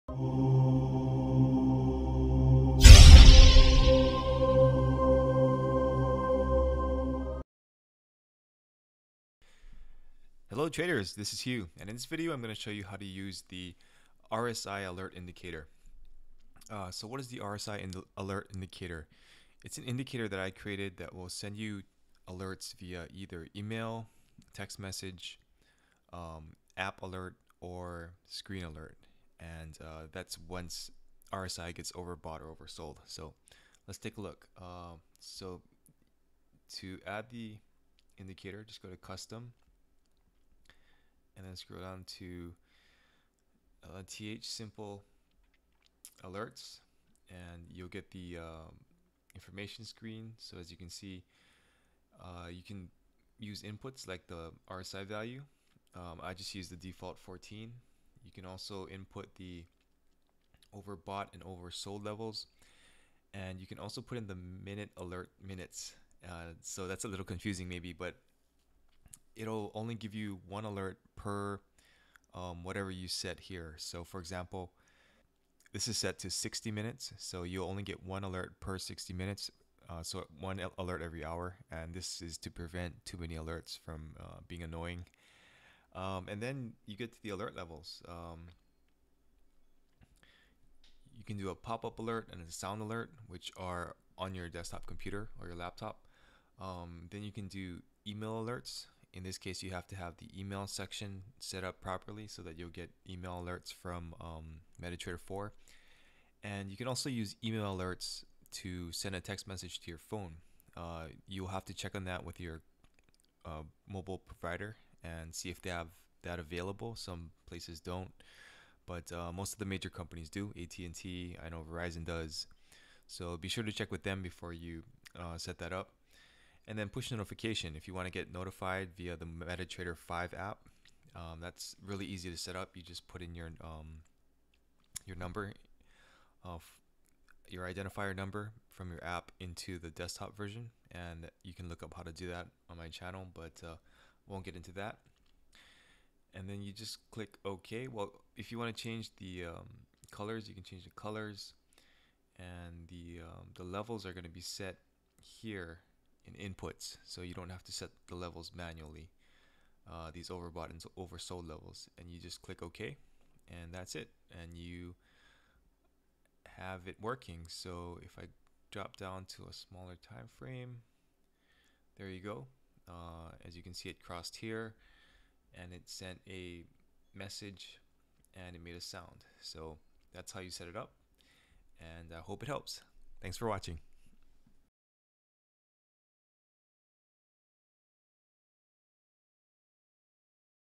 RSI Alerts Indicator for MT4: Trading Heroes RSI
Learn how to setup the MT4 RSI Alerts indicator, so you get alerts via smartphone, text message, email or screen alert.

When the Relative Strength Index is overbought or oversold, you will get an alert on one more of the notification methods you choose in the settings.

Nostr: npub1v5ty092cnhtt6aehn0csu9mhfk0yprqz8335ae0y2y07fk95vmmqrn26pk